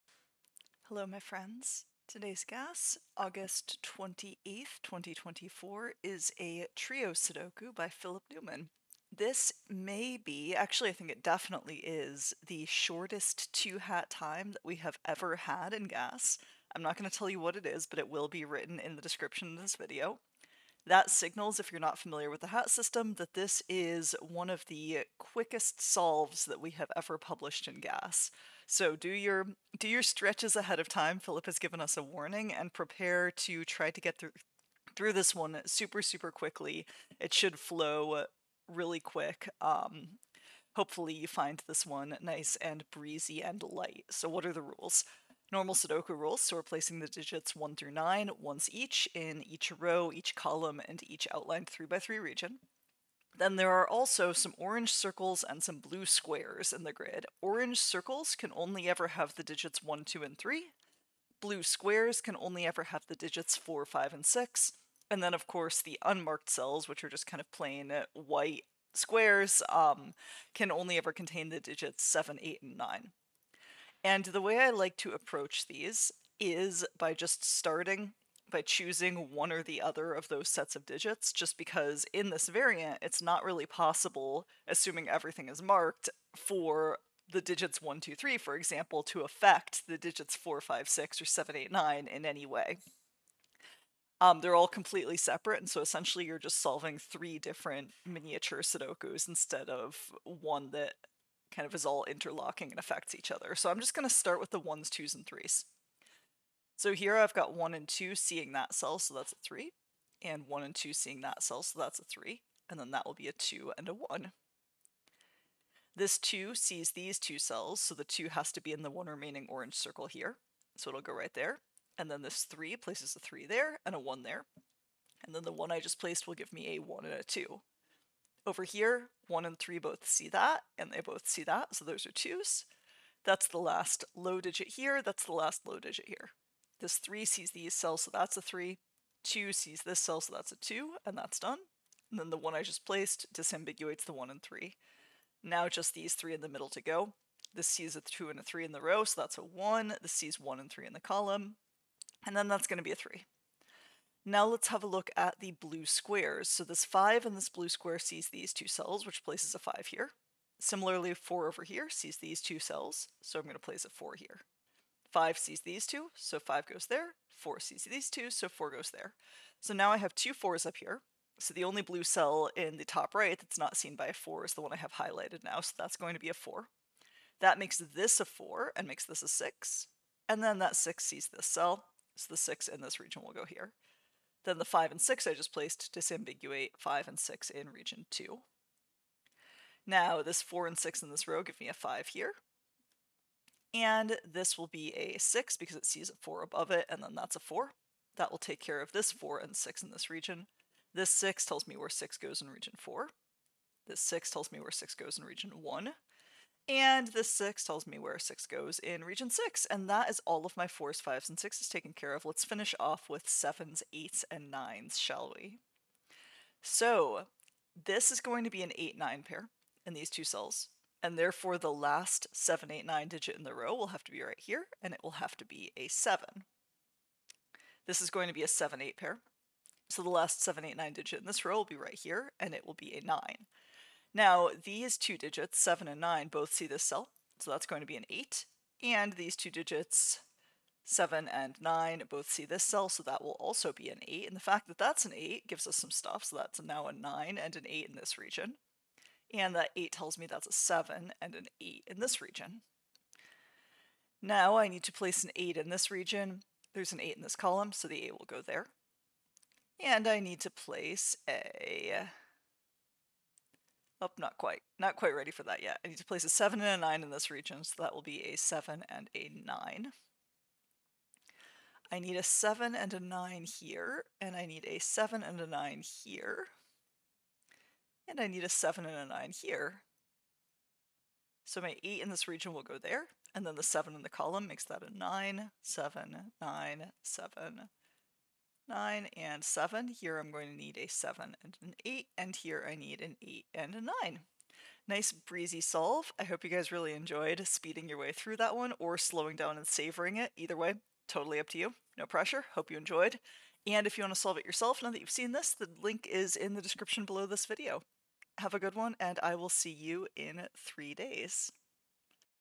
GAS Sudoku Walkthrough - Trio by Philip Newman (2024-08-28)
Clover solves Philip's sudoku "Trio", originally posted on August 28, 2024.

RULES:
Trio: The digits 1, 2, and 3 are marked with orange circles; the digits 4, 5, and 6 are marked with blue squares.

HAT TIMES:
3:00/10:00